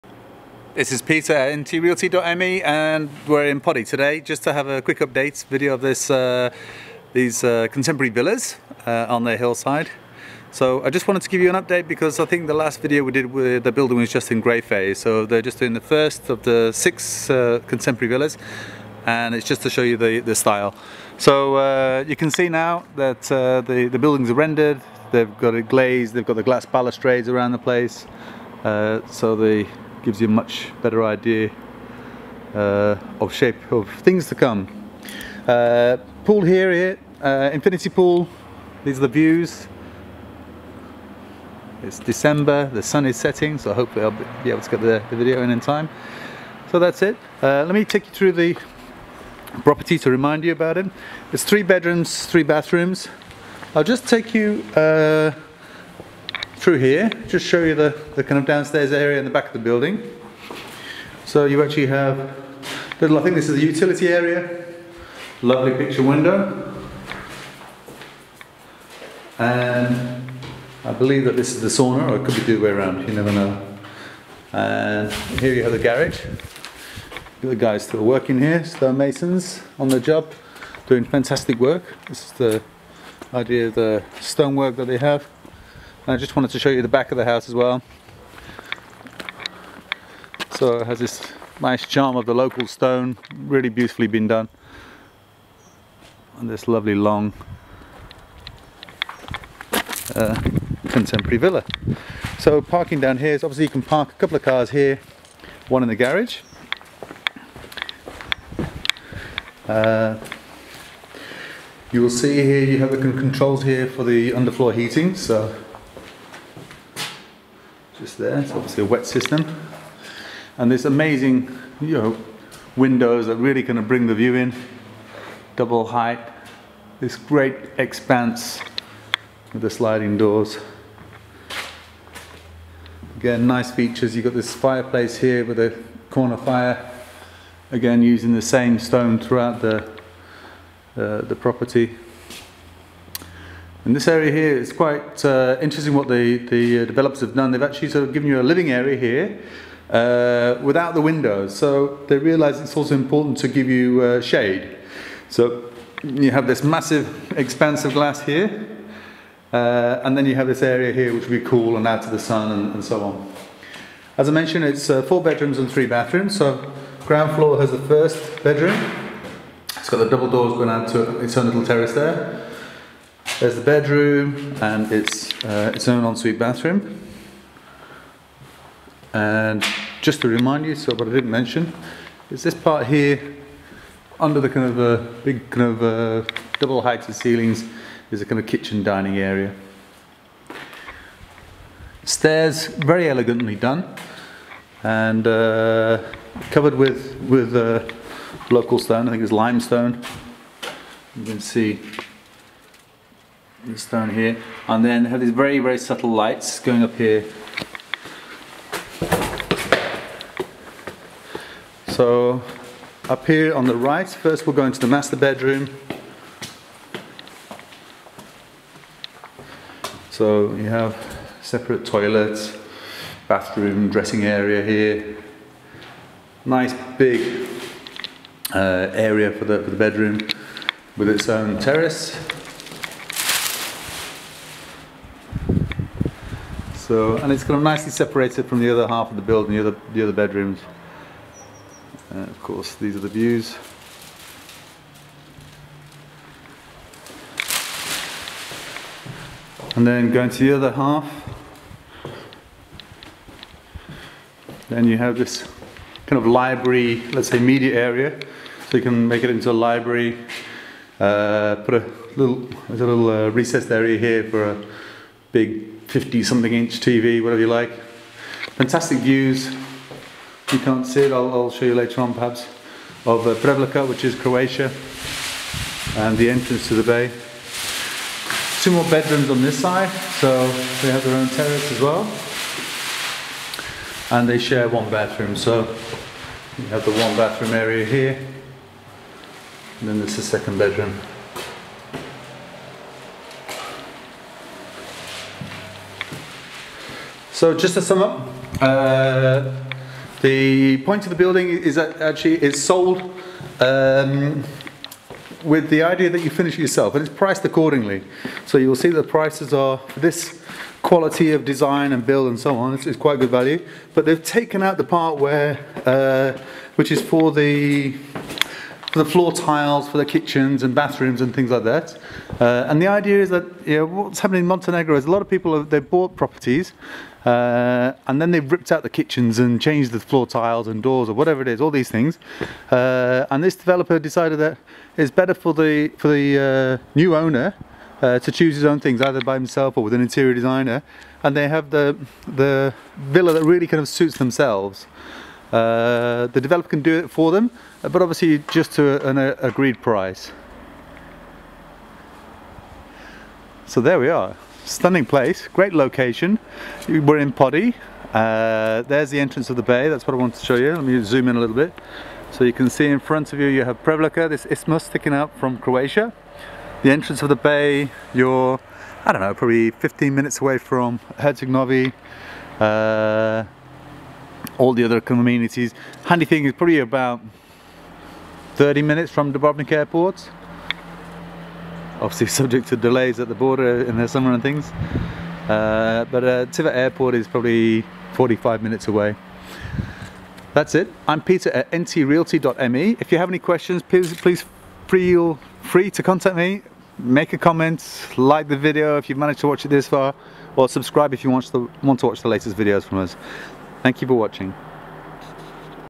SOLD Herceg Novi Bay - Podi, Contemporary Villas
An absolutely stunning contemporary 4 bedroom, 3 bathroom villa above the picturesque village of Podi with breath taking views overlooking the entrance to the Boka Bay and Prevlaka Fort, Croatia.

Within a gated complex of six private residences, the villa is designed for luxury living. The expansive floor-ceiling windows are designed to maximise the view of the Bay and the generous living space is combined with large terraces and swimming pool with an amazing infinity edge view out over the Bay. Garage, a sauna room and hot tub are amongst the many features that mark this as an outstanding property.

The concept by the developers is to build stunning villas which can be finished to your own liking and therefore the costs of kitchen bathrooms, flooring and internal decoration have been removed from the overall price, allowing you to personalise your villa while at the same time not wasting time and money redoing and paying for things that don't necessarily suit your individual tastes.

The villa is located at 250m above sea level, the air-line (as the crow flies) distance from the coast less that a kilometre. The ten minute drive to the newly built Marina Meljine gives you the opportunity to take a boat and set on the high seas.  Of course being in the Boka Kotorska (Boka Bay) you are surrounded by interesting places to visit and delicious food in the waterside restaurants. The nearby town of Herceg Novi has great night life and a board walk that stretched for miles (or kilometres if you prefer).

Midway between the airports Dubrovnik and Tivat, this villa is conveniently located enabling you to use it as a quick getaway retreat at any time of the year.

Main Features:

· Size of the plot: 878 m2

· Gross build area: 355,64 m2

· 2 levels

· Net build area: 245,13 m2

· Pool net area: 43,0 m2

· Base of the object: 192,32 m2 (Gross Build Area)

· Base of the pool: 49,12 m2 (Gross Build Area)

· Outside space: 636,56 m2

Ground Floor

· Large entrance hall

· Sweeping central stairs

· 1st bedroom

· Large double height living room with sliding doors to a large terrace with pool

· Lounge with glass fronted fireplace

· Spacious kitchen and dining area

· Sauna and shower room

· Utility room

· Garage

First Floor

· Master bedroom with large balcony, walk-in wardrobe and generous en-suite bathroom

· Galleried library/office/play zone

· 3rd bedroom with balcony

· 4th bedroom with balcony

· Family bathroom

Landscape Gardens and Facade

· The facade of this architect designed villa is as you’d expect complimentary with the use of local stone and glass

· The elongated 40m2 infinity pool allows you to enjoy laps or gaze at the views of Lustica Peninsula and Prevlaka Peninsula (Croatia)

· The hot tub allows you the same wonderful views as the pool but is even more relaxing

· The sunbathing area is decked with wood

· Barbecue area

· Gardens planted with palm and olive trees

Widows and Technologies

· Energy efficient double glazed windows with aluminium frames (Alumil S700 Supreme, M300 sliding doors and M11000 Alutherm Plus

· Built in Electric roller shutters

· Eco-Friendly VRV system (heating pumps) for heating and cooling

· Water based under-floor heating system

· Cooling via integrated ceiling air-conditioning/fan coils

· Centralised HVAC with the additional features of underfloor heating (wet) in the kitchen and bathrooms

Shell and Walls

· Steel reinforced anti-seismic concrete frame structure

· 30cm thick clay thermo bricks

Indoor Finishing

· Interior floors are prepared for tiles or wooden flooring throughout

· Walls and ceilings are prepared for painting

· Bathrooms with built-in Geberit toilets, waterproof walls and floors prepared for tiling

· Bespoke doors, frames, trims

· Stone covered stairs with glass railings

Interior works are done to the level of preparation for final craft works, which offers the possibility of complete control of the interior design specified and built you your own standards and tastes. This is of course at your own expense, but the developers can assist with your bespoke interior design fulfillment.

Tivat Office: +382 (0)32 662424
Mobile (En): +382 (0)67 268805
Mobile (En,Mne) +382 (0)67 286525
Skype 1: newterritory4u
Skype 2: ntrealty4u